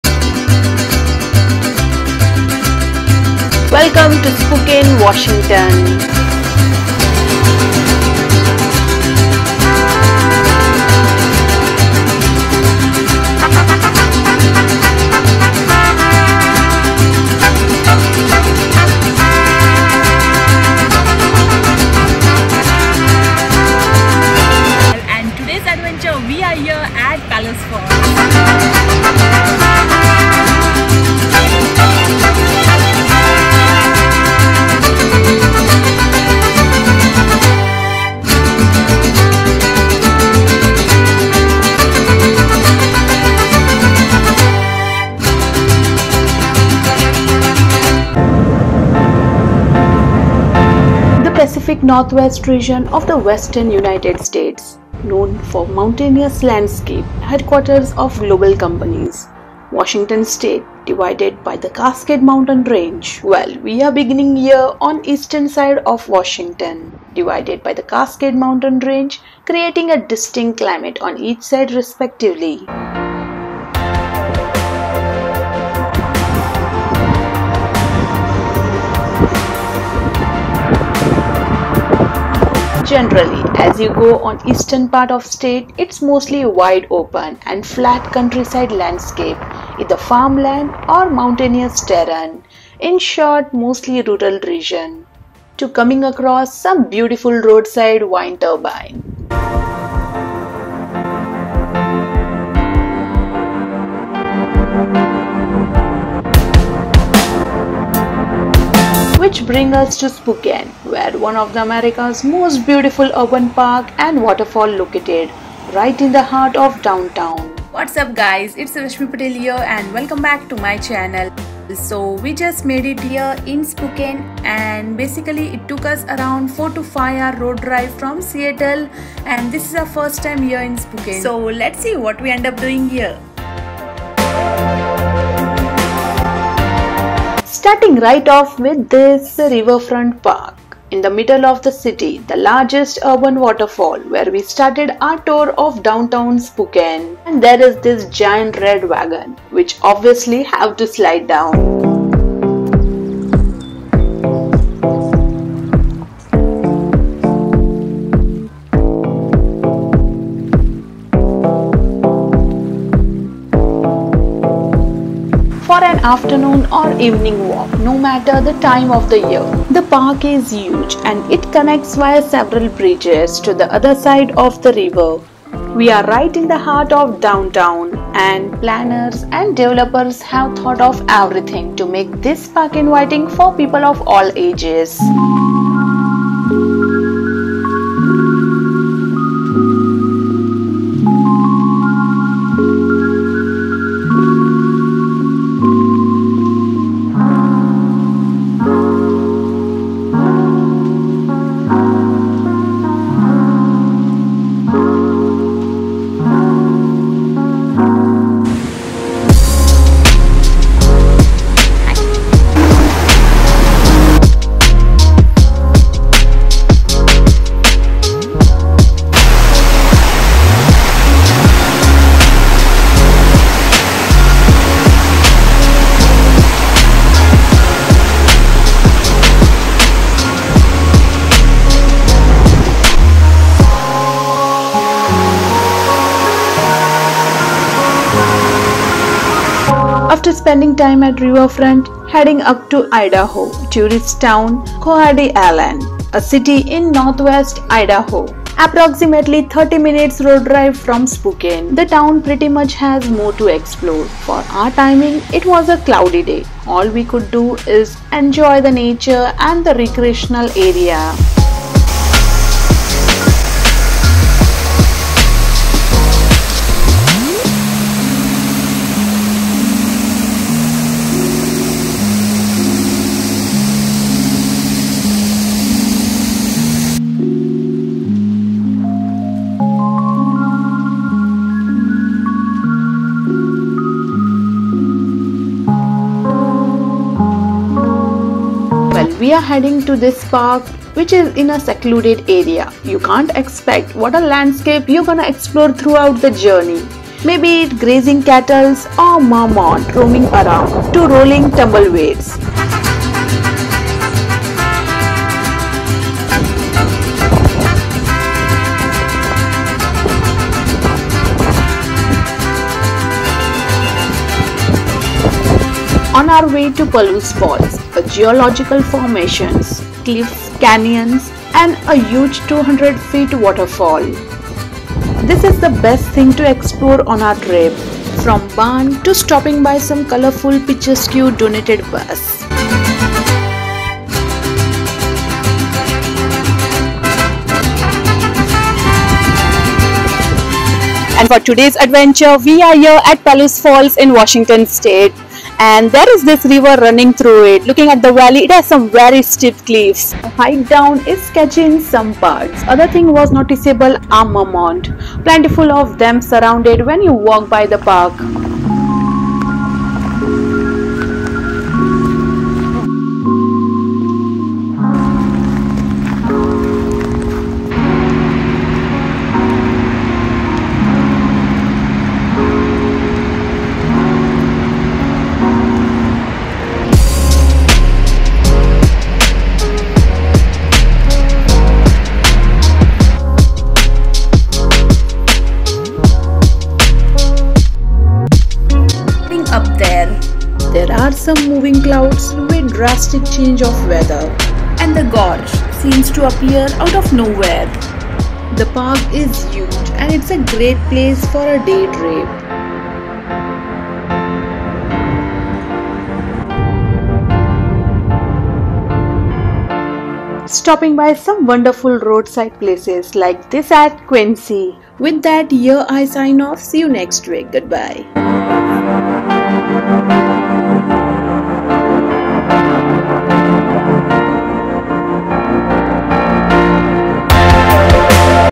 Spokane ,Palouse falls & Idaho Vlog 2023 | A road trip to Idaho
Hello guys, my name is Rashmi Patel. I make videos related to Travel, Lifestyle & DIY every week.
In this video Join us to a road trip from Seattle to Idaho and what not in between from Palouse Falls to Spokane .A secluded place, especially heading towards Pallouse Falls. Also the sacred Spokane waterfront .